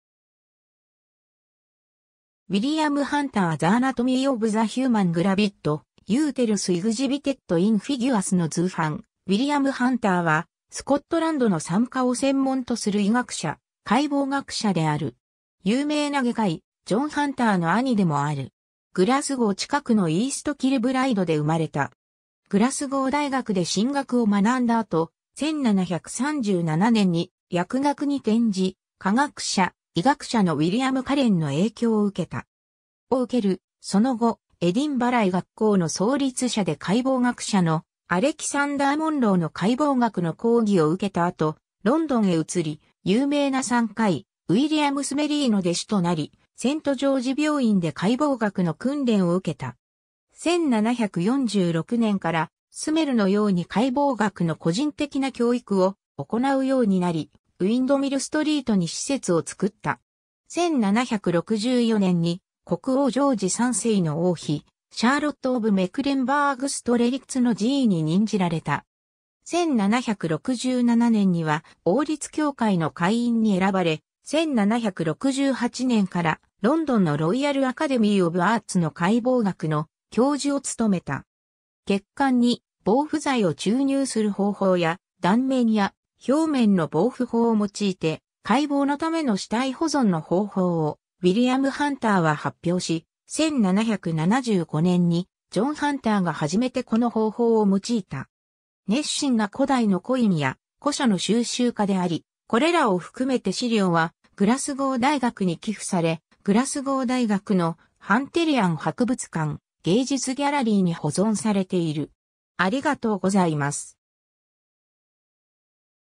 ウィリアム・ハンター (医学者)
イースト・キルブライド出身の人物
William Hunter The anatomy of the human gravid uterus exhibited in figuresの図版 ウィリアム・ハンター（William Hunter 、1718年5月23日 - 1783年3月30日）は、スコットランドの産科を専門とする医学者、解剖学者である。
有名な外科医、ジョン・ハンターの兄でもある。
 グラスゴー近くのイースト・キルブライドで生まれた。
グラスゴー大学で神学を学んだ後、1737年に薬学に転じ、化学者、医学者のウィリアム・カレンの影響を受けた。
（1750年にグラスゴー大学で学位（M.D.）をうける）、その後、エディンバラ医学校の創立者で解剖学者のアレキサンダー・モンロー（Alexander Monro I. :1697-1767）の解剖学の講義を受けた後、ロンドンへ移り、有名な産科医、ウイリアム・スメリーの弟子となり、セント・ジョージ病院で解剖学の訓練を受けた。
 1746年からスメルのように解剖学の個人的な教育を行うようになり、ウインドミル・ストリートに施設を作った。
1764年に国王ジョージ3世の王妃、シャーロット・オブ・メクレンバーグ＝ストレリッツの侍医に任じられた。
1767年には王立協会の会員に選ばれ、1768年からロンドンのロイヤル・アカデミー・オブ・アーツの解剖学の教授を務めた。
 血管に防腐剤を注入する方法や、断面や表面の防腐法を用いて解剖のための死体保存の方法を、ウィリアム・ハンターは発表し、1775年にジョン・ハンターが始めてこの方法を用いた 。
 熱心な古代のコインや古書の収集家であり、これらを含めて資料はグラスゴー大学に寄付され、グラスゴー大学のハンテリアン博物館・芸術ギャラリー（Hunterian Museum and Art Gallery）に保存されている。